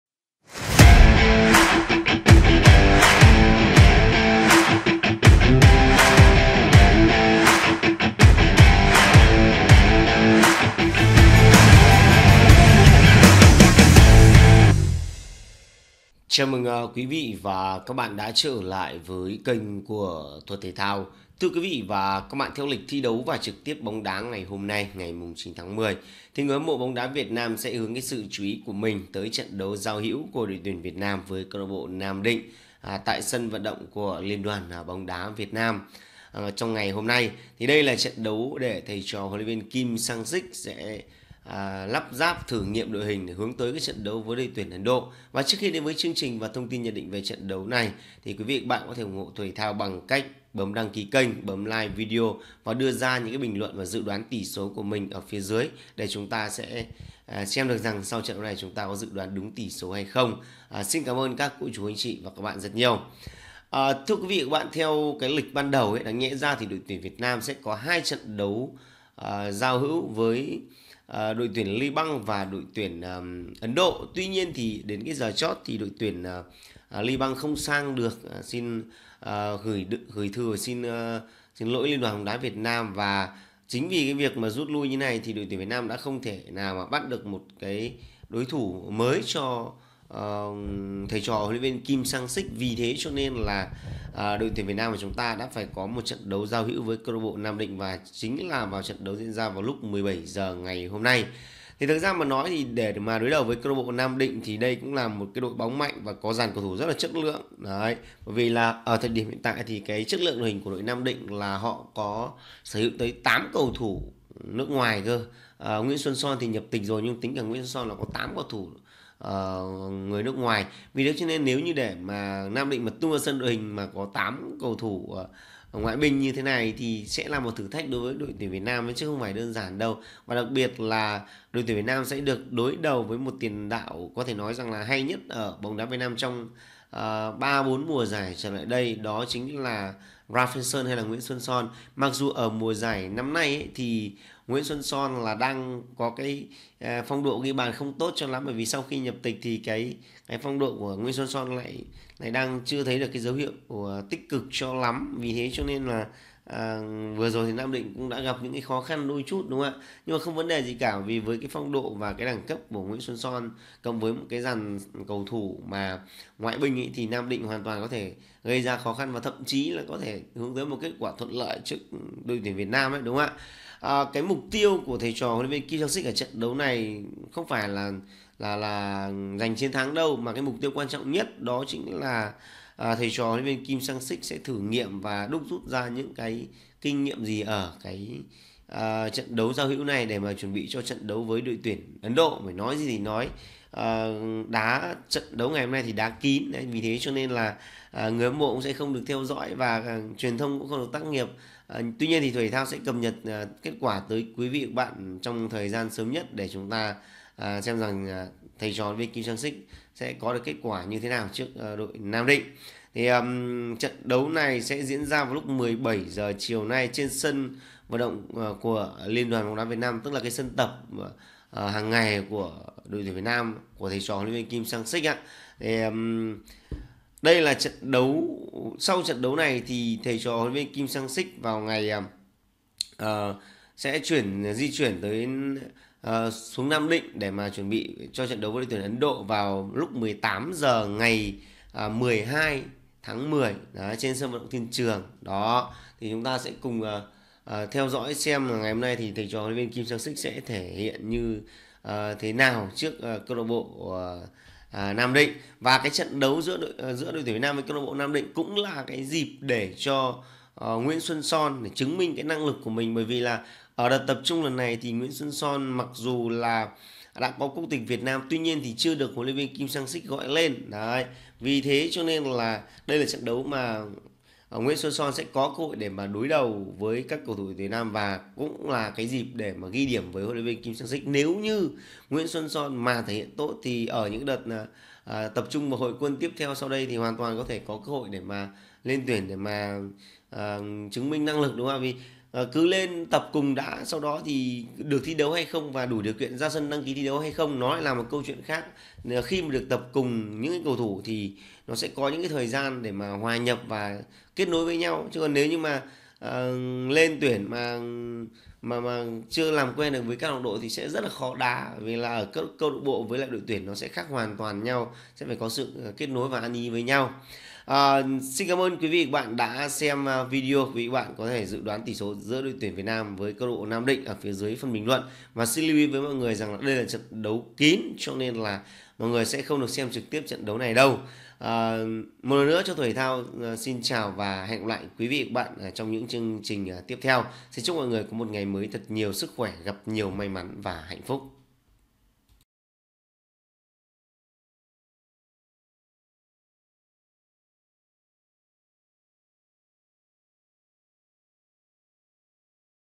Lịch trực tiếp bóng đá hôm nay 9-10 | ĐT Việt Nam vs CLB Nam Định 17h trên sân VFF
✔ Trang youtube chính thức của nhà báo Dương Thuật